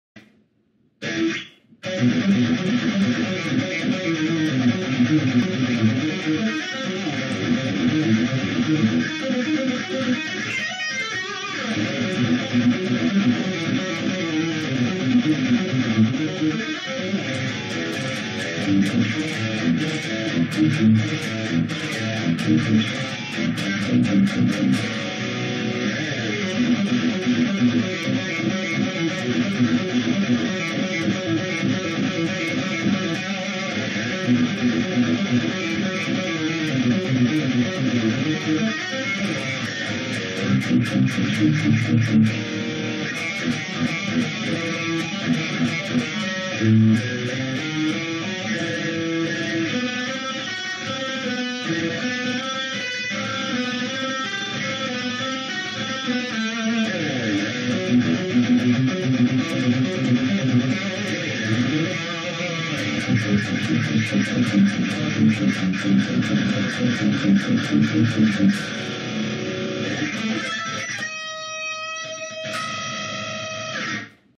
Technical Deathcore idea
when I'm bored, I play many styles of music, this is a short demonstration of an idea of extreme metal

I use Standard Tuning  E, A, D, G, B, and E